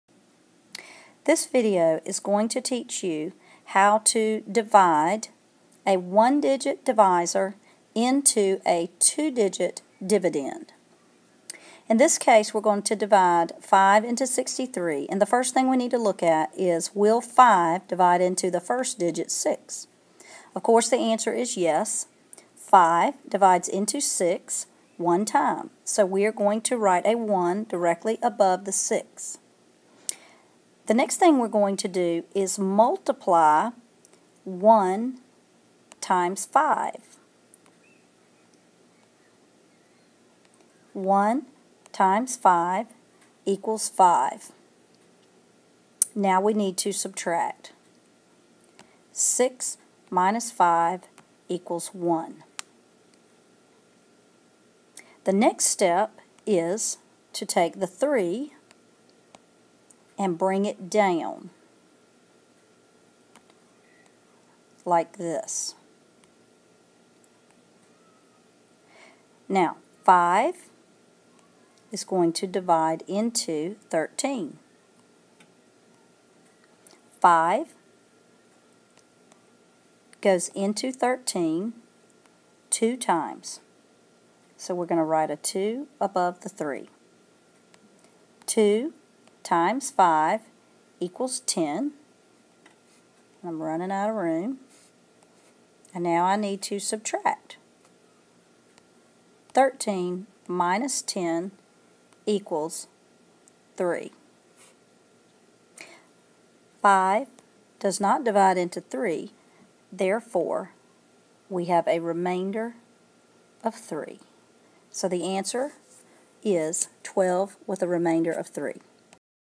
Long division 2digit/1digit w/ Remainders
Long division of 2 digit dividend by a 1 digit divisor